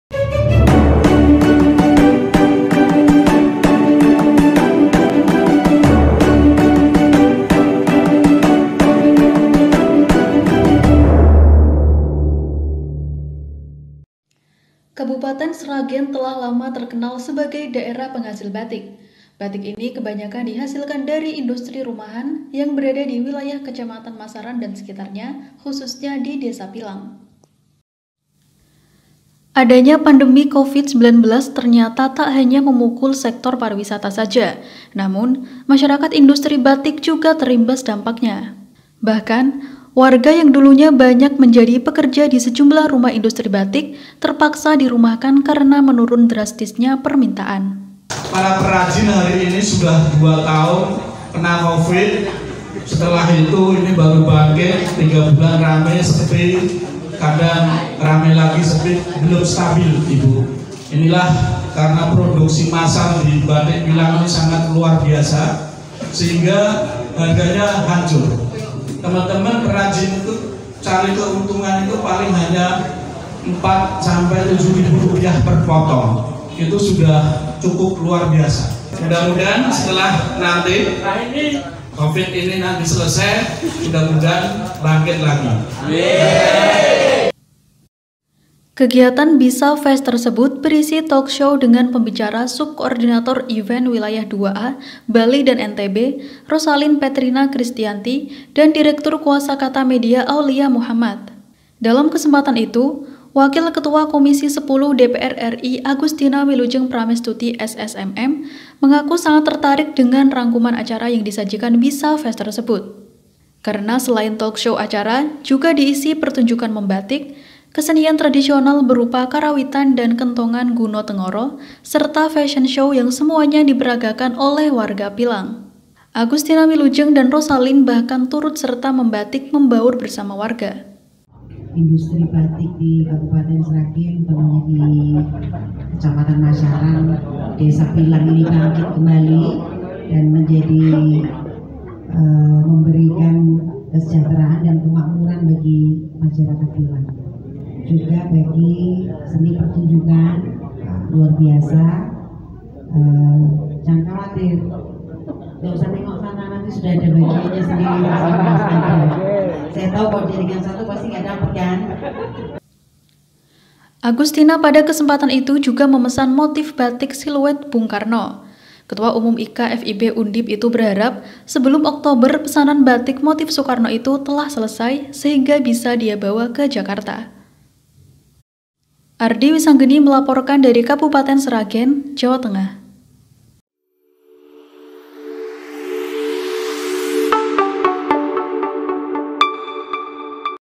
Sempat Terpuruk Dihantam Covid, Kini Batik Pilang Sragen Mencoba Bangkit
Kabupaten Sragen telah lama terkenal sebagai daerah penghasil batik, batik ini kebanyakan di hasilkan dari indsutri rumahan yang berada di wilayah Kecamatan Masaran dan sekitarnya, khususnya di Desa Pilang.

Namun, adanya pandemi Covid-19 ternyata tidak hanya memukul sektor pariwisata saja namun masyarakat industri batik juga terimbas dampaknya. Bahkan, yang dulu sebelum pandemi banyak warga yang menjadi pekerja di sejumlah rumah industri batik banyak dirumahkan karena menurun drastisnya permintaan.

Kegiatan BISA Fest tersebut berisi talk show dengan pembicara Sub Koordinator Event Wilayah II A (Bali dan NTB) Rosalin Petrina Kristianti dan Direktur Kuasa Kata Media Aulia Muhammad.

Dalam kesempatan itu, Wakil Ketua Komisi X DPR RI Agustina Wilujeng Pramestuti SS, MM, mengaku sangat tertarik dengan rangkuman acara yang disajikan BISA Fest tersebut, karena selain talk show acara juga diisi pertunjukkan membatik, kesenian tradisional berupa karawitan dan Kentongan Gunotengoro, serta fashion show yang semuanya diperagakan oleh warga Pilang.

Agustina Wilujeng dan Rosalin bahkan turut serta membatik membaur bersama warga.

Agustina pada kesempatan itu juga memesan motif batik siluet Bung Karno. Ketua Umum IKA FIB Undip itu berharap, sebelum Oktober pesanan batik motif Sukarno itu telah selesai, sehingga bisa dia bawa ke Jakarta.